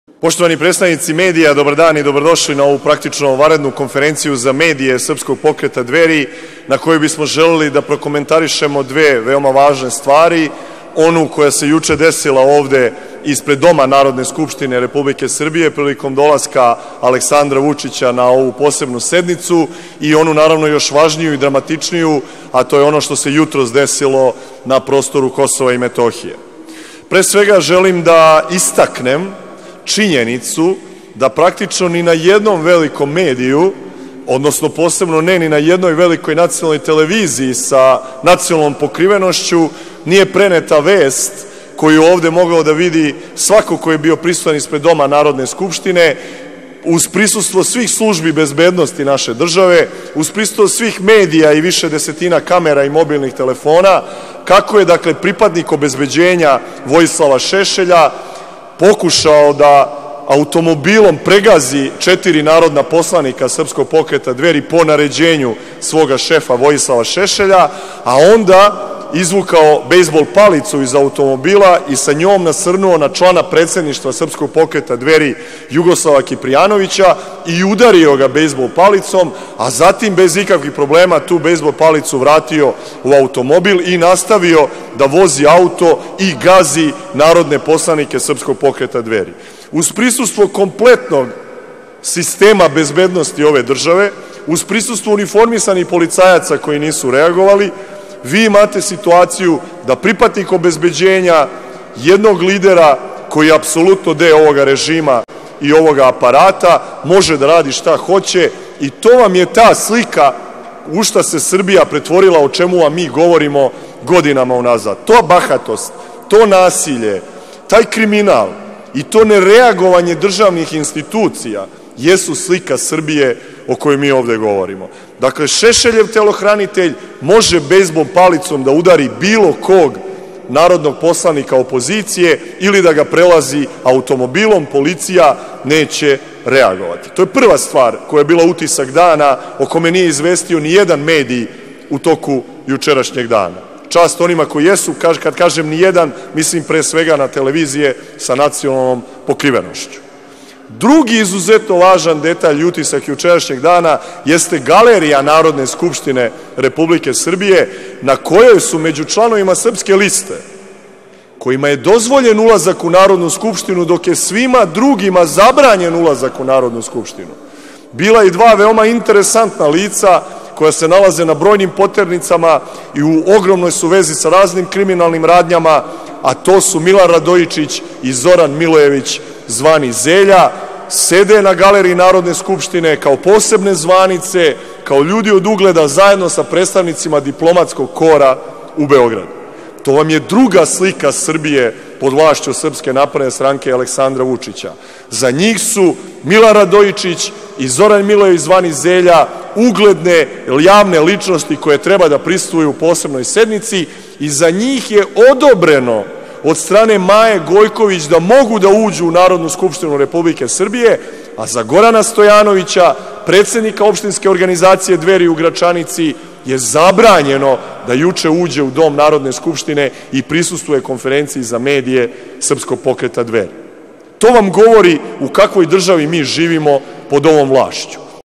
SLIKA SRBIJE - Boško Obradović
Boško Obradović govori o pravoj situaciji u Srbiji. Nijedan medij sa nacionalnom frekvencijom nije preneo sukob koji se odigrao ispred skupštine.